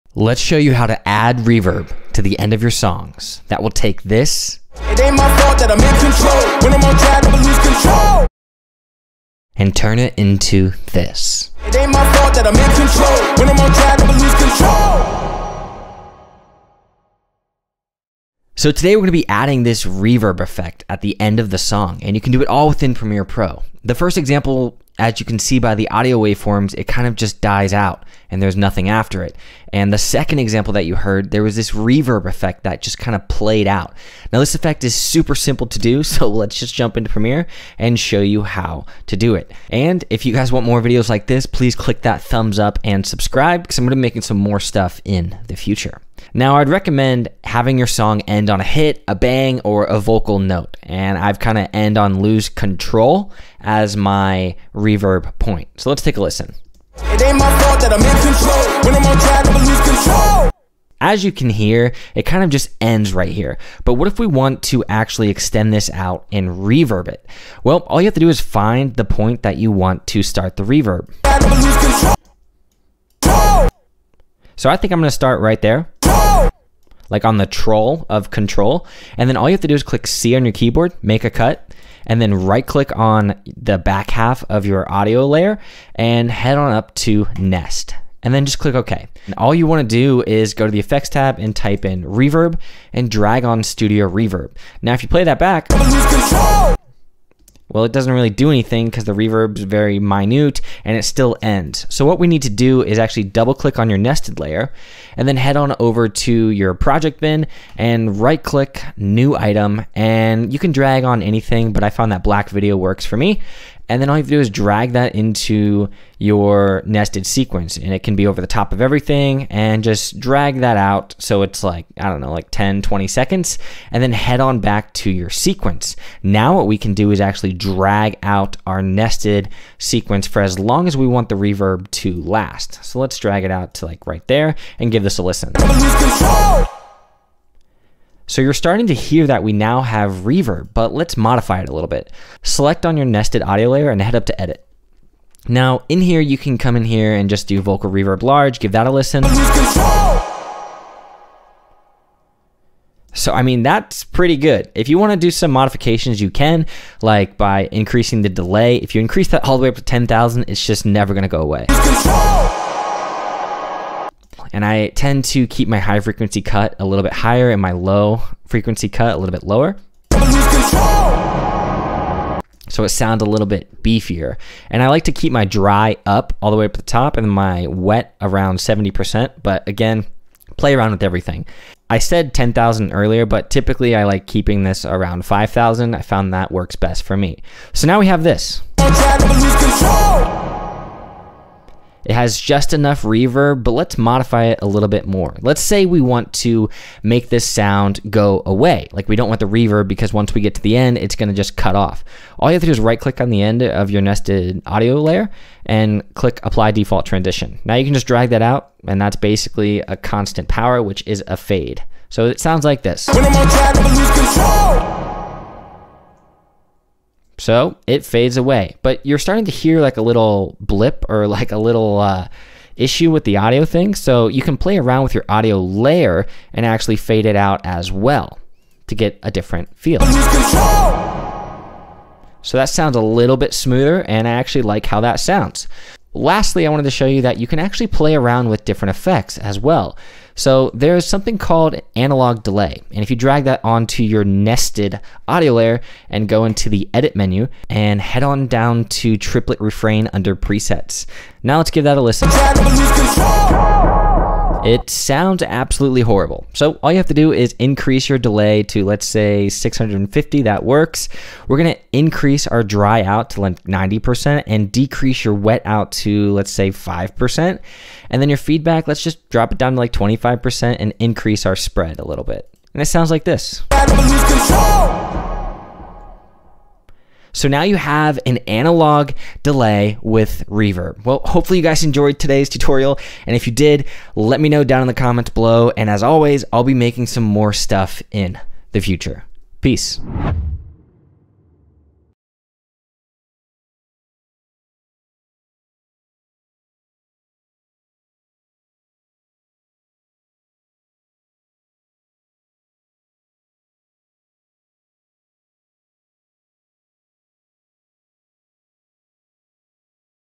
End a Song with REVERB | Premiere Pro
Have you ever wanted to just end a song but it just doesn't work? Let's show you how to add reverb and delay to the end of your music!

👀 CHAPTERS 👀
0:00 | INTRO
0:18 | WHAT IS THE REVERB EFFECT?
0:40 | TUTORIAL START
1:40 | HOW TO MODIFY NEST AUDIO LAYER
2:35 | MODIFY REVERB EFFECT
3:15 | MAKE THE REVERB FADE AWAY
3:55 | ADD ECHO EFFECT
4:37 | CLOSING THOUGHTS